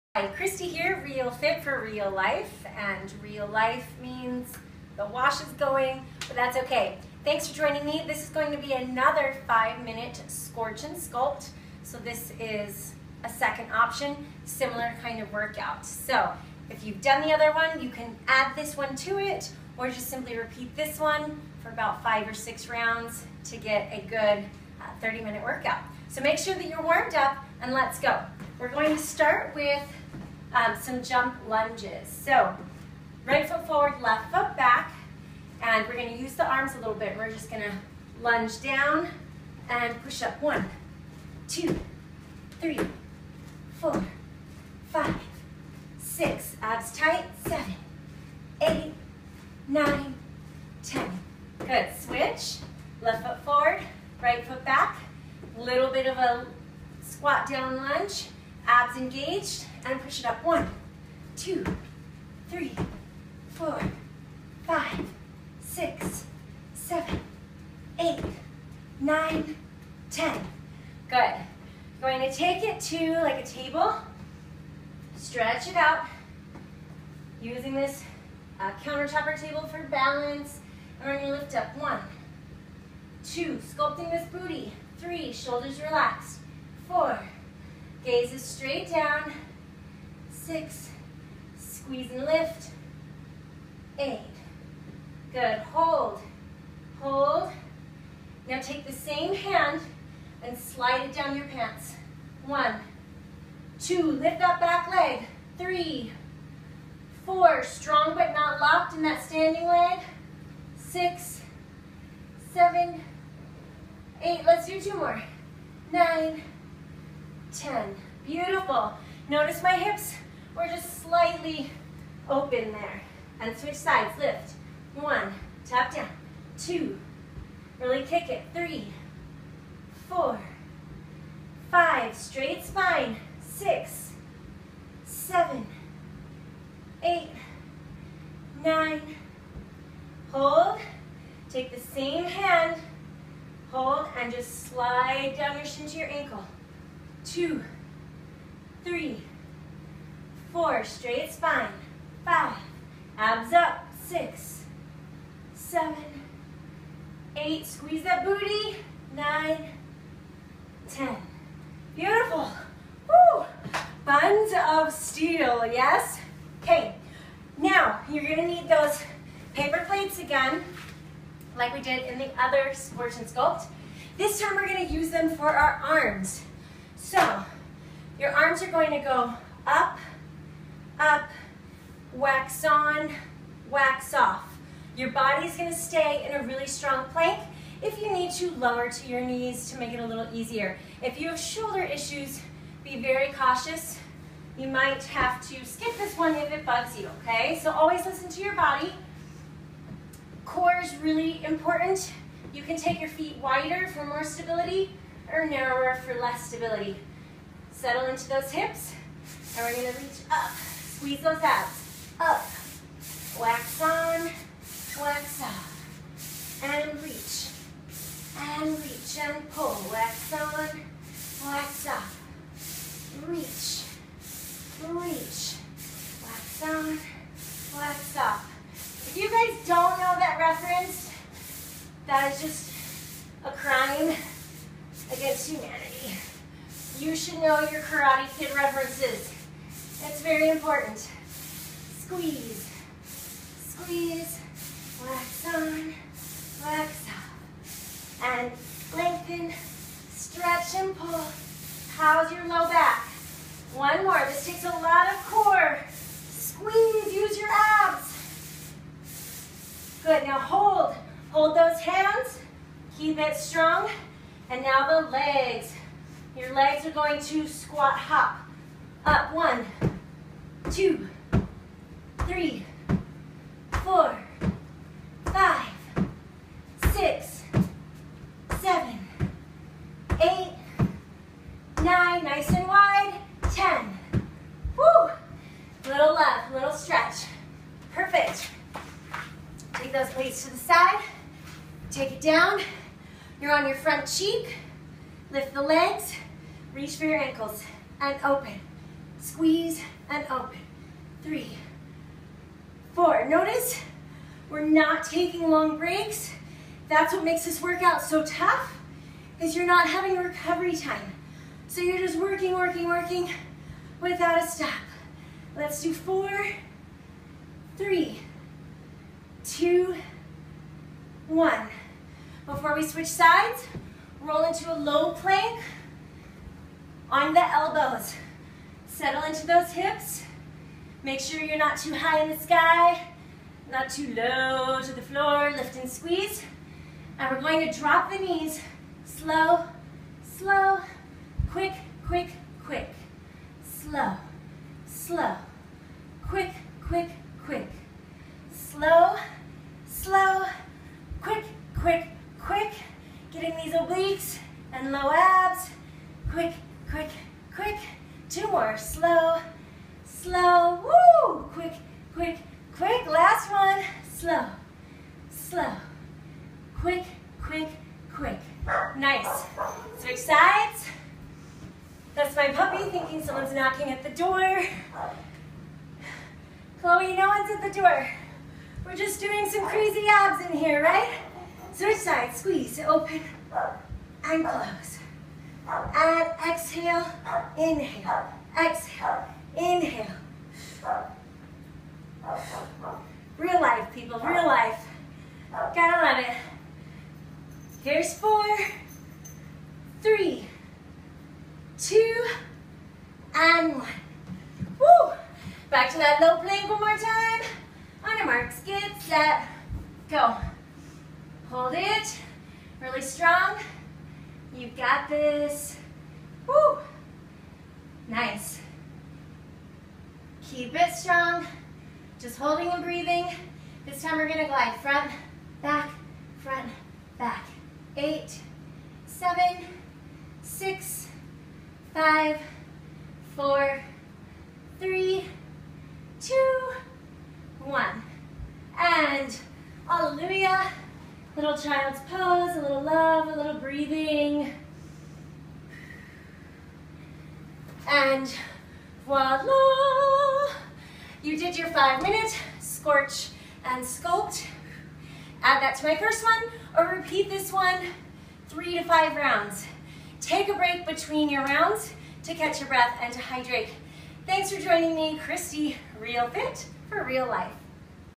5-minute Scorch & Sculpt #2
This workout is designed to scorch fat & sculpt your whole body! Repeat multiple rounds or mix and match with other Christy workouts to create an effective, fun routine that fits any schedule!!